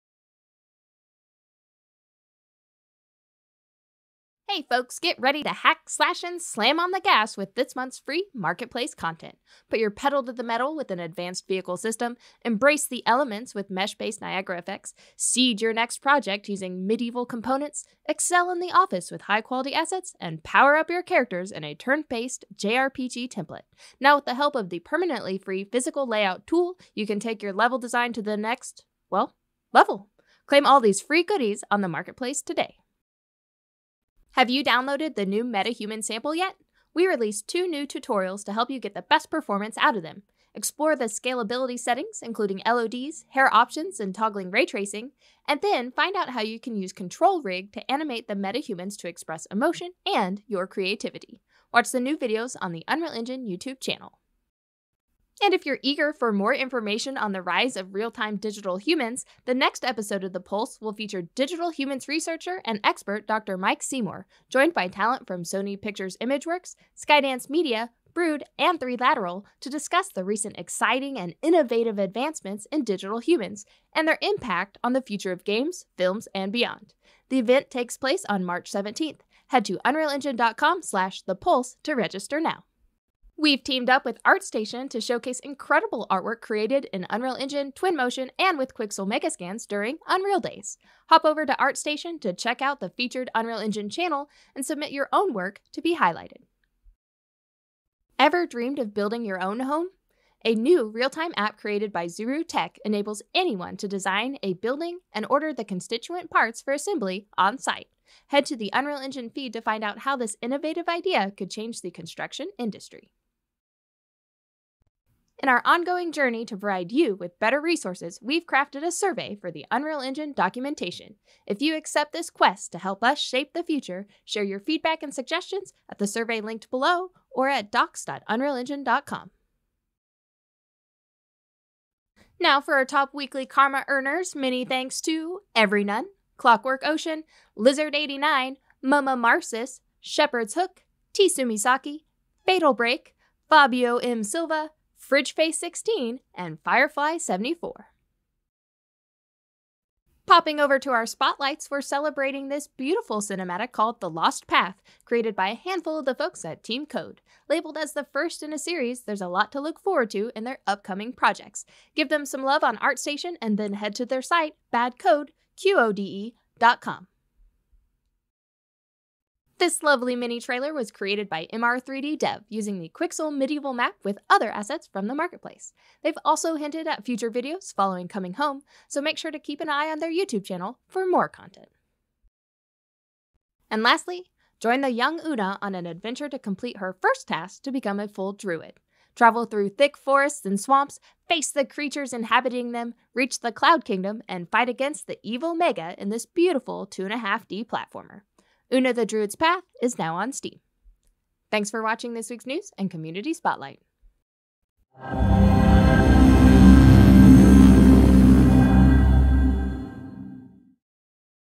News and Community Spotlight | March 4, 2021 | Unreal Engine
NEWS

Meet the real-time BIM app for designing and constructing buildings

COMMUNITY SPOTLIGHTS